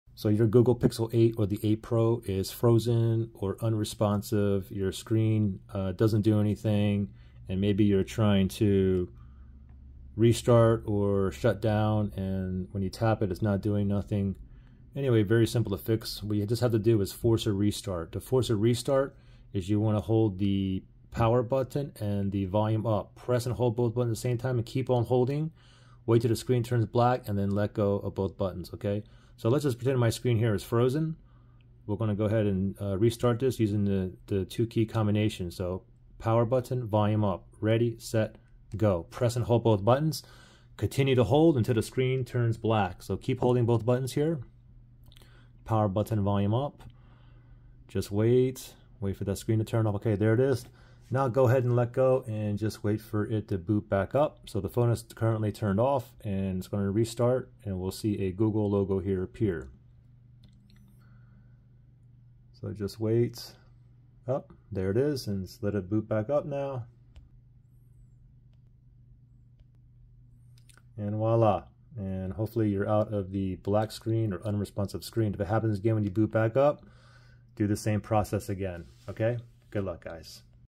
Pixel 7 & 8: Screen is Frozen, Unresponsive, or Stuck? FIXED!
I show you how to fix a screen that is frozen, unresponsive, stuck somewhere like in app or on Google Logo on the Google Pixel 8 & 8 Pro  (same for Pixel 7 & 7 Pro) smartphones. I hope this helps.

Google Pixel 8 - Unlocked Android Smartphone
Google Pixel 8 Pro - Unlocked Android Smartphone